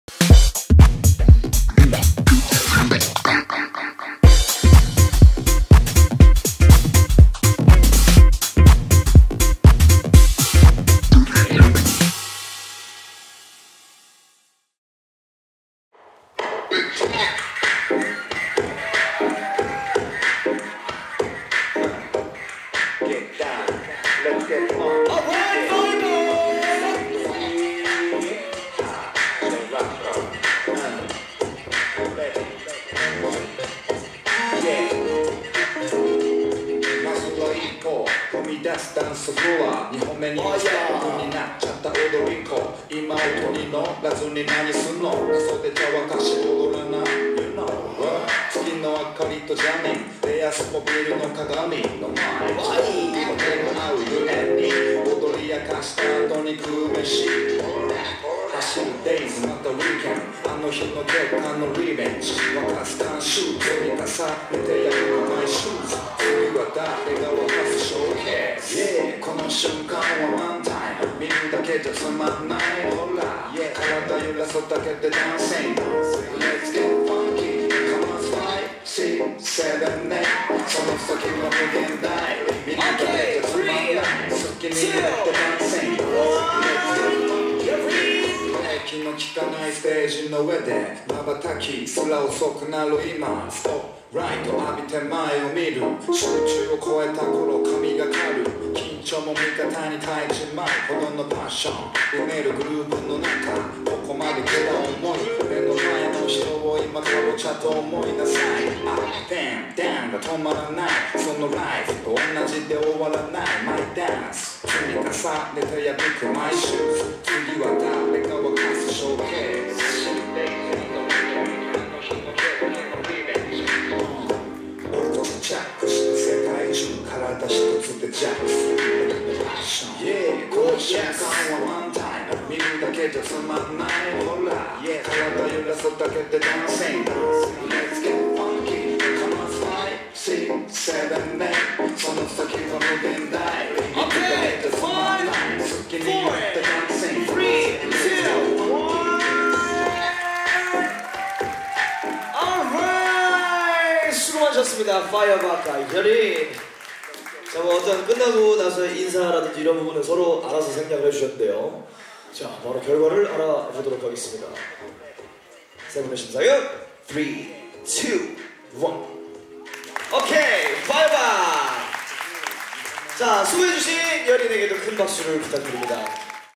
FIRE BAC vs YEORINㅣPOPPING Quarter Final ㅣ2019 LINE UP SEASON 5
2019 LINE UP SEASON 5 ANNIVERSARY

Quarter FinalㅣFIRE BAC (w) vs YEORIN

💎POPPING💎
🎧DJ
•SATOCI
👑JUDGE
•POPPIN ZERO •POPPIN J •HOZIN
♟BATTLEGUEST
•TAI •EUN G •FIREBAC •JAYGEE

2019/5/11 - 12
@ARCO CONERT HALL(57PARTY TOWN)_GWANGJU CITY

Present by BITGOEUL DANCERS

VIDEO BY 'HAMONG'